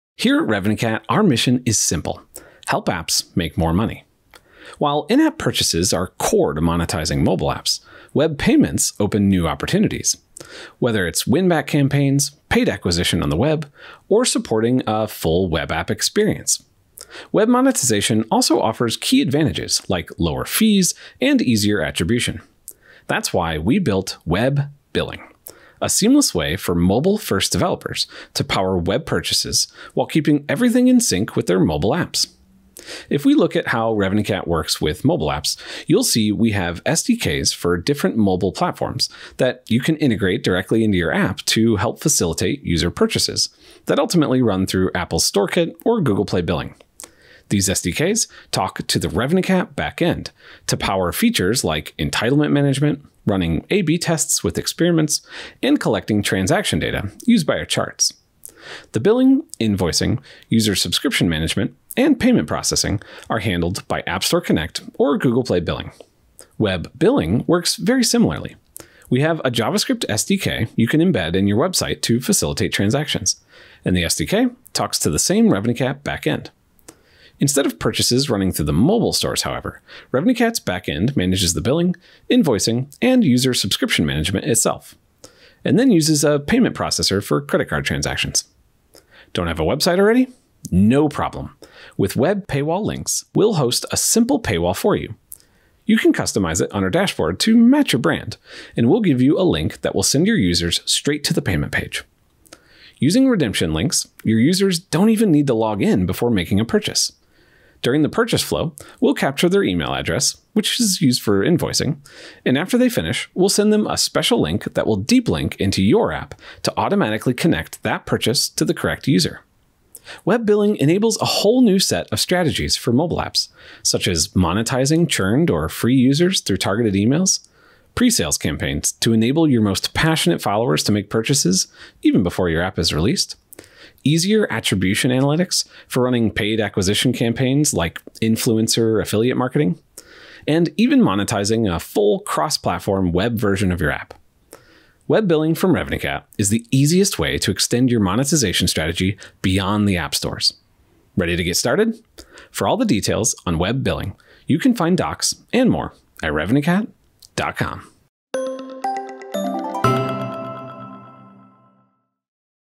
Introducing Web Billing
Web Billing from RevenueCat is the easiest way for mobile devs to unlock the flexibility of web purchases —and make more money. No coding or hosting required. Getting started with web is as easy as sharing a link.